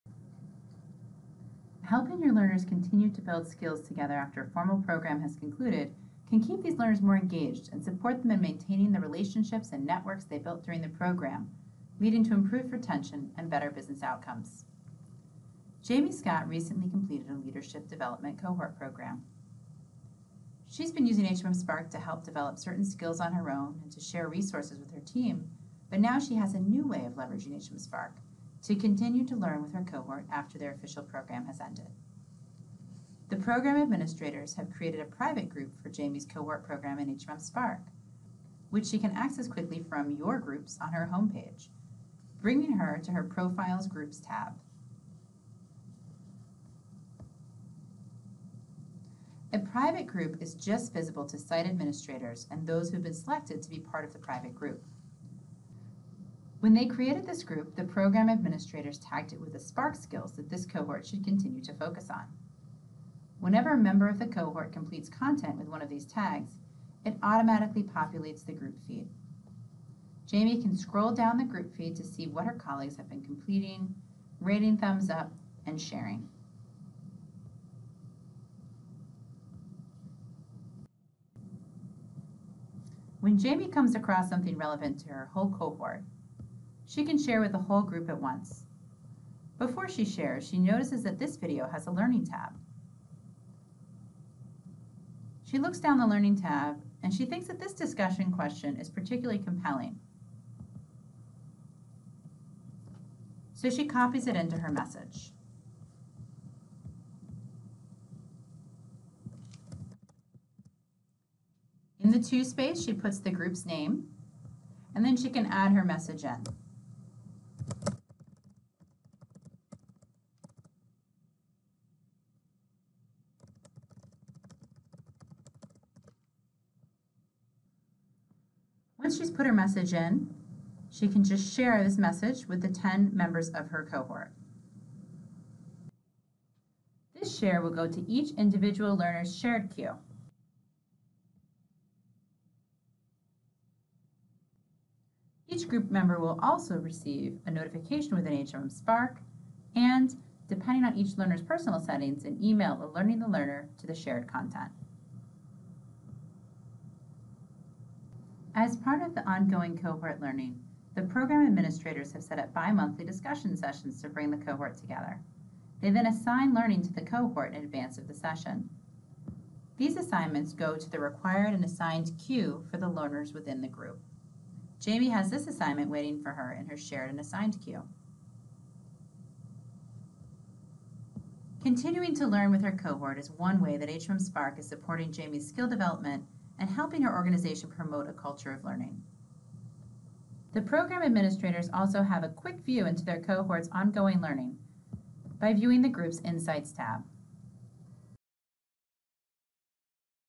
Ongoing Cohort Learning in HMM Spark
How to keep your learners engaged with group activities